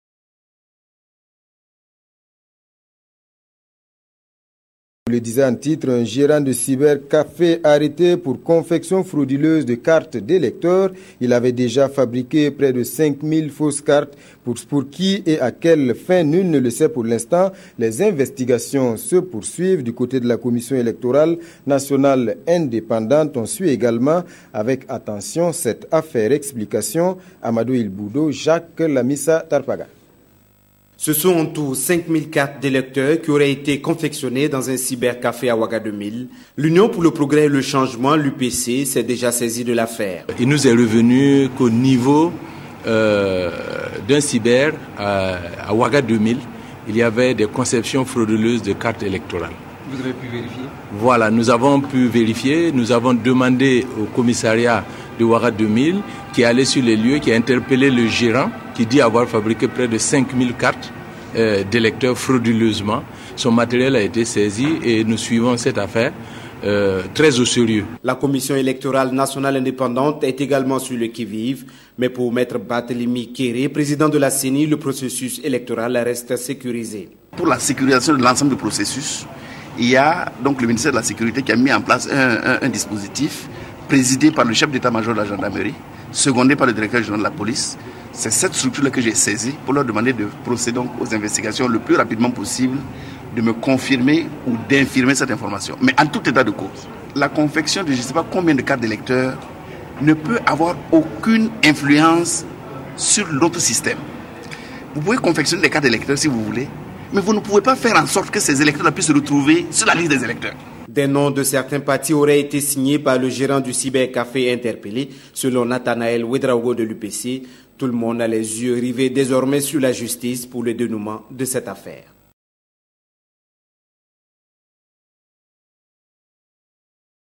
RTB / Un gérant de cyber aux arrêts pour confection frauduleuse de cartes d'électeurs
Un gérant de cyber aux arrêts pour confection frauduleuse de plus 5000 cartes d'électeurs